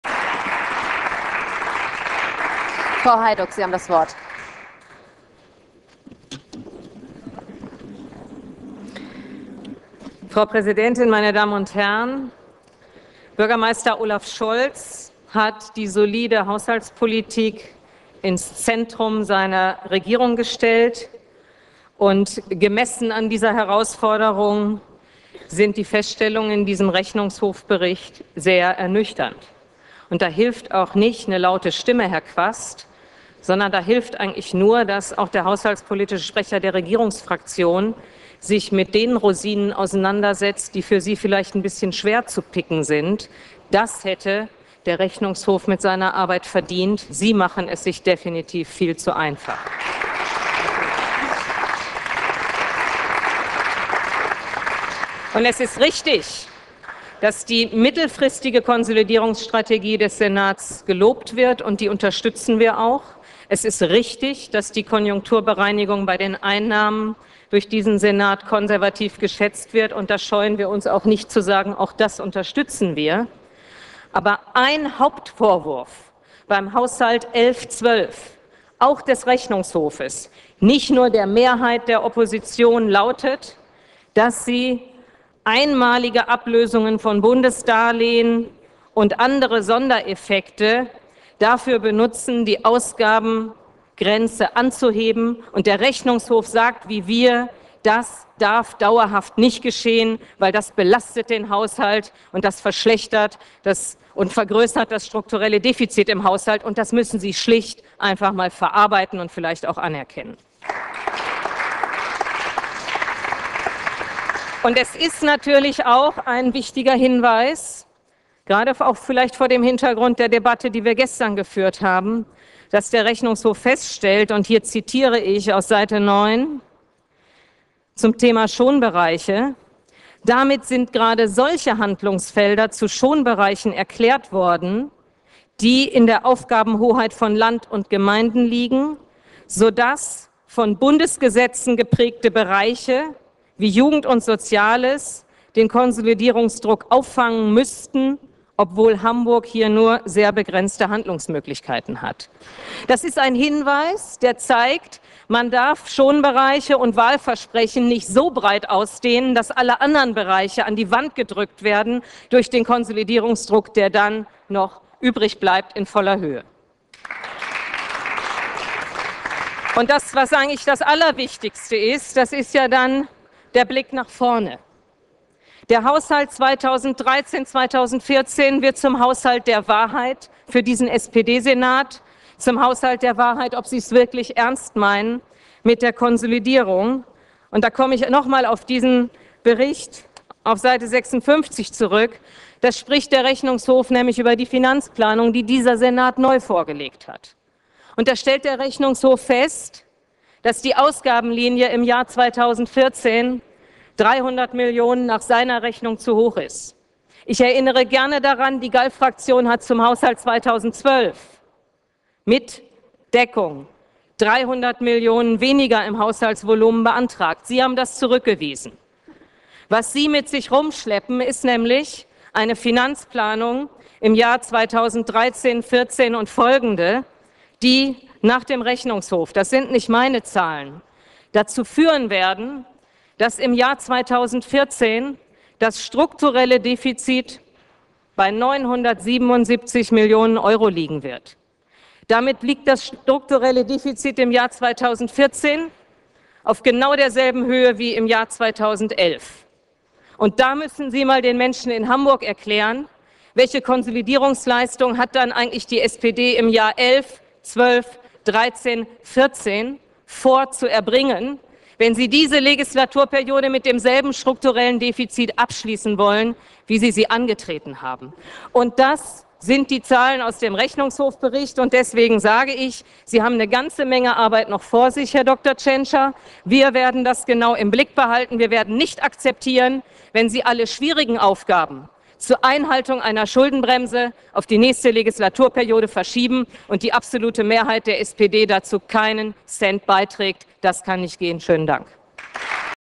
Anja Hajduk, MdHB, zum SPD-Haushalt
In der Sitzung der Hamburgischen Bürgerschaft am 9. Februar 2012 hat die haushaltspolitische Sprecherin und stellv. Vorsitzende Anja Hajduk in der Aktuellen Stunde zur SPD-Haushaltspolitik gesprochen.